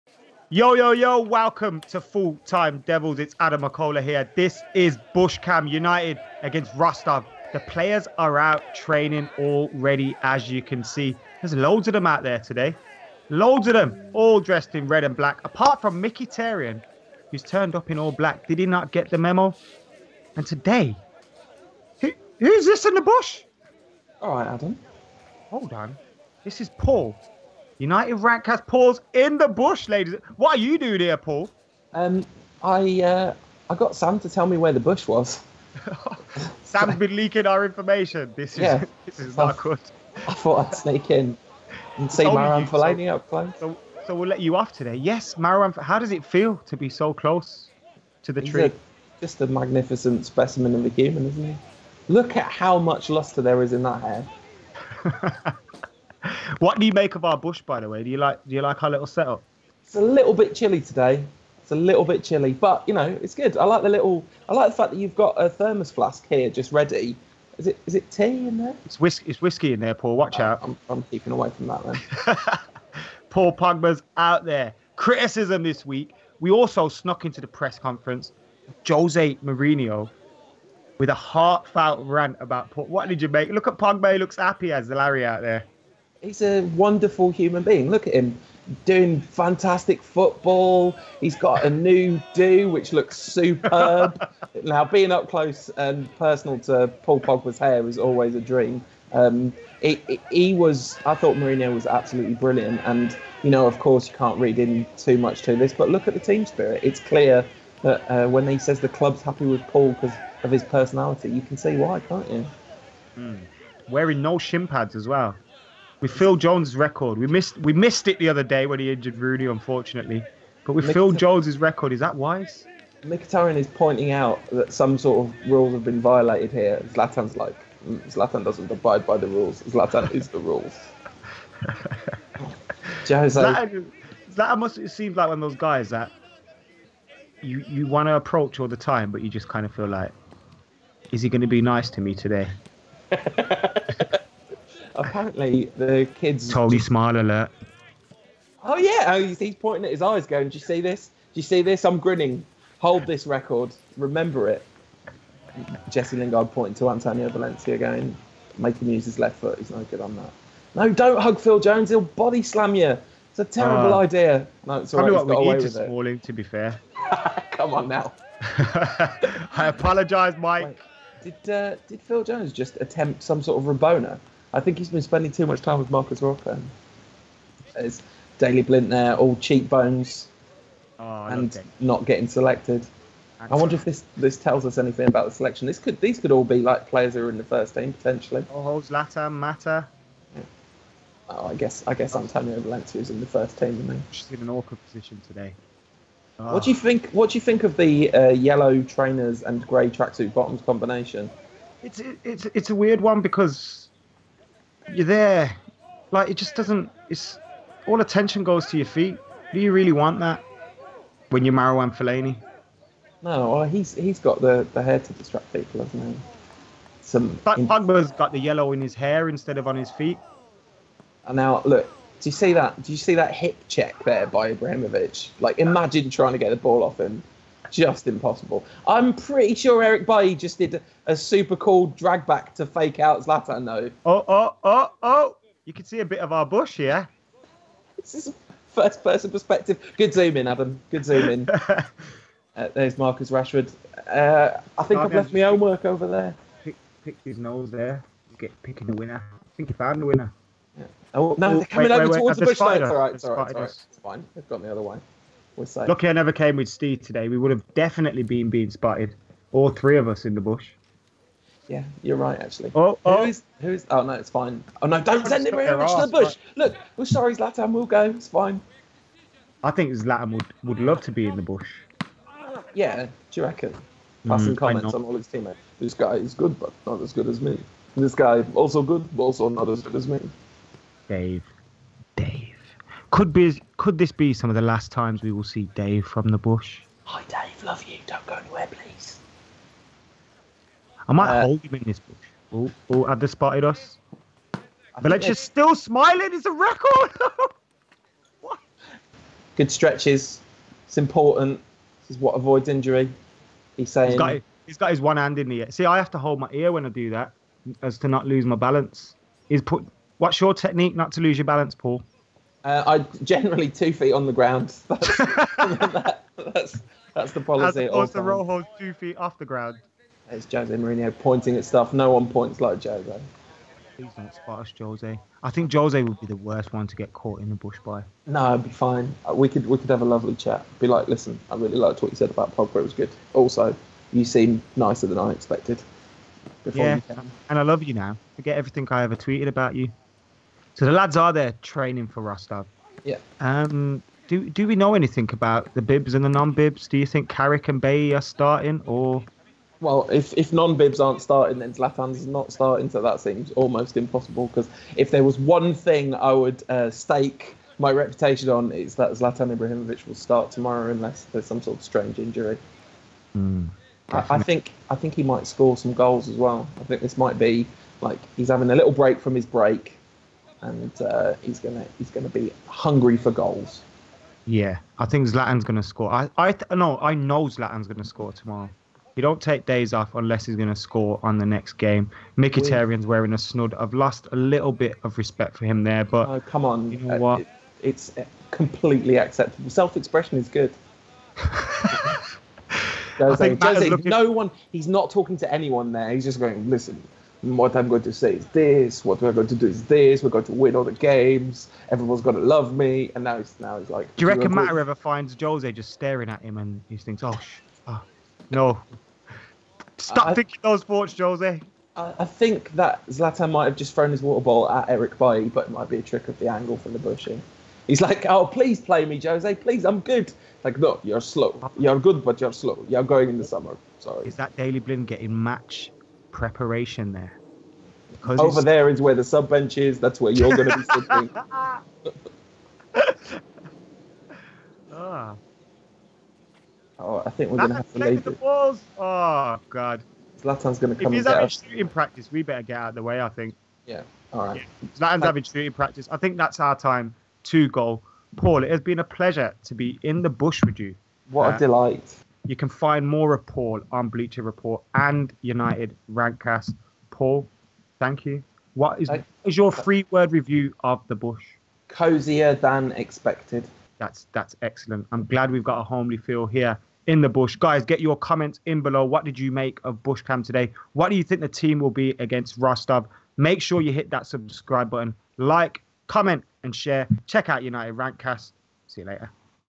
BUSHCAM: Manchester United vs FC Rostov | TRAINING EXPOSED!
Manchester United training exposed! It could only be BUSHCAM!!! Ahead of the Europa League Last 16 second leg match vs FC Rostov!